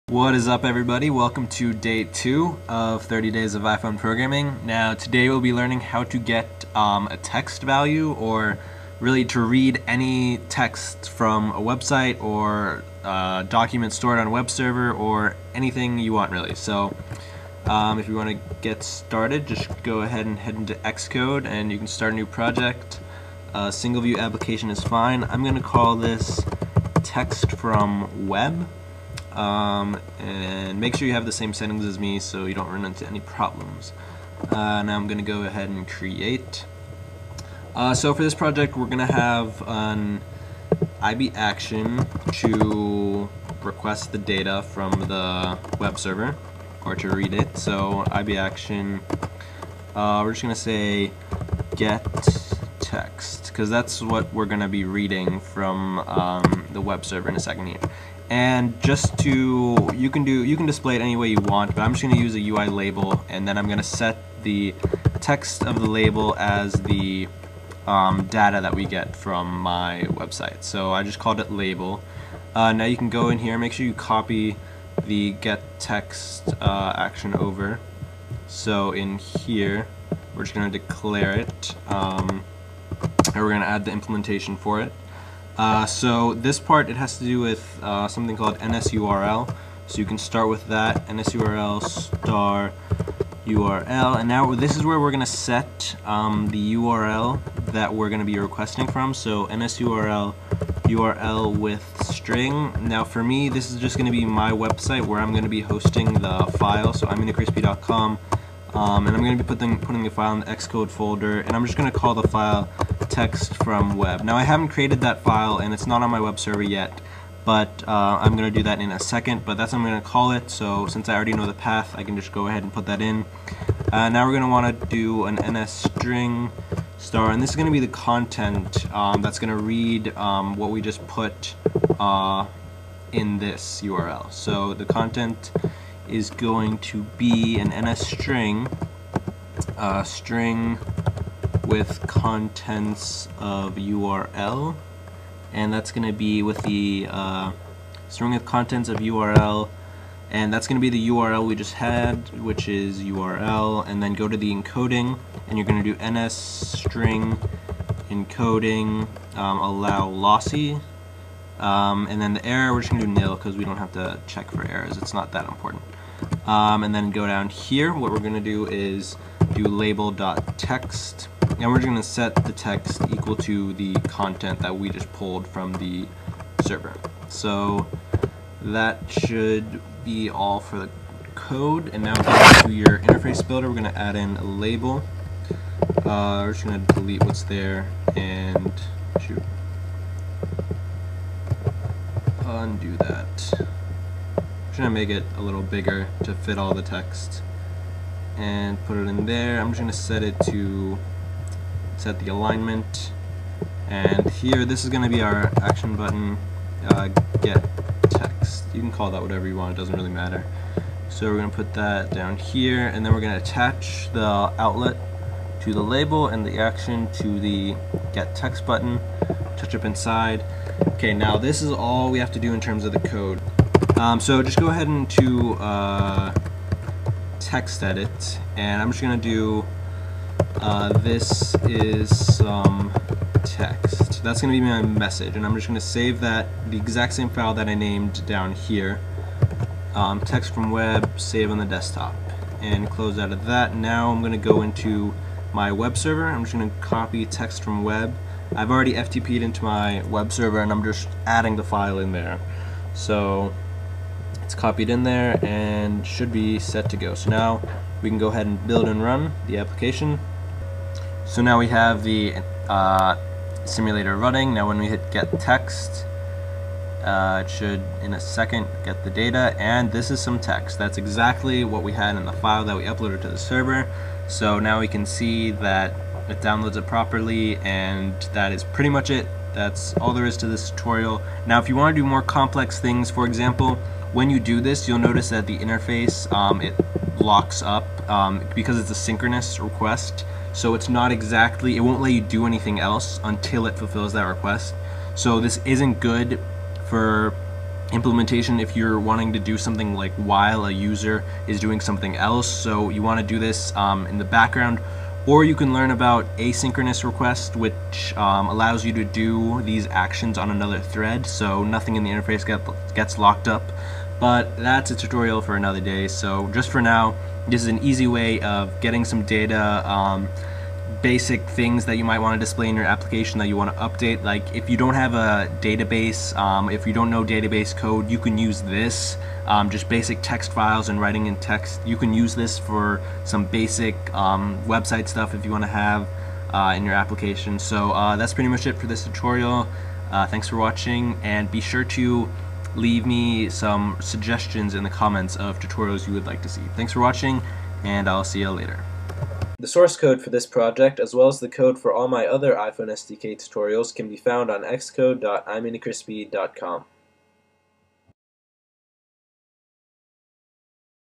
[Xcode] Text From Web
This tutorial will show you how to read basic text from files uploaded to the web and how to display this text in an application.